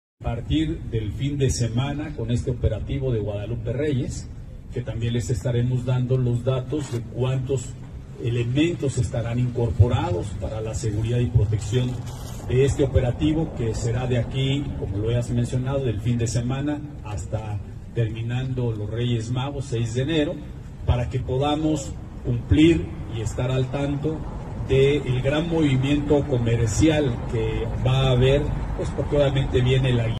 Eduardo Rivera informó que a partir de este fin de semana dará inicio el Operativo Guadalupe-Reyes
El alcalde Eduardo Rivera, informó que a partir de este fin de semana dará inicio el Operativo Guadalupe-Reyes para brindar seguridad a la ciudadanía en esta temporada navideña